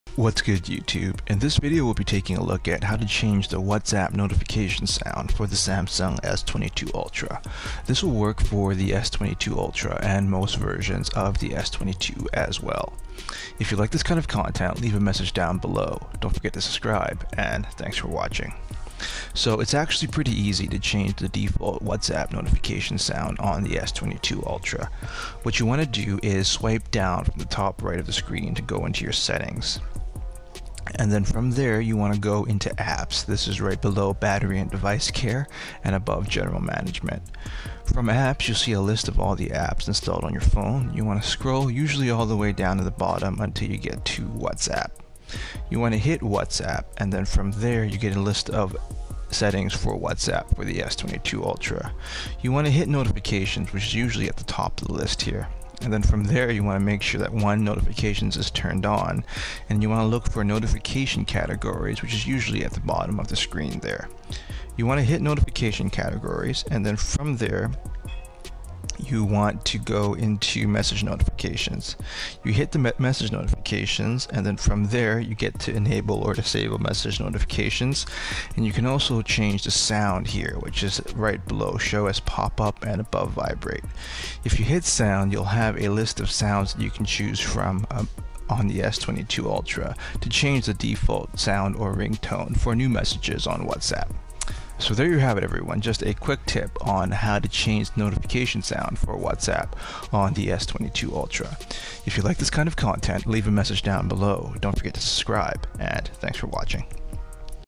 Samsung S22 Ultra - How to change the Whatsapp notification sound.
This video will go over who to change the Whatsapp notification sound on the Samsung S22 Ultra.

S22 Ultra Cases